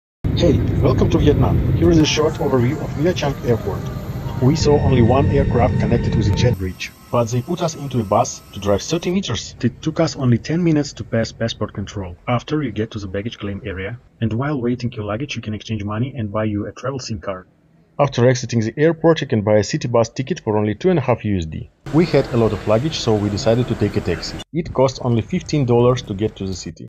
of Nha Trang airport ! Our first big Vietnam trip started.
We arrived to Vietnam.
ready for any collaboration.
also photoshots or any other activities.

We can make you photoshot, any review of your tourist business, etc.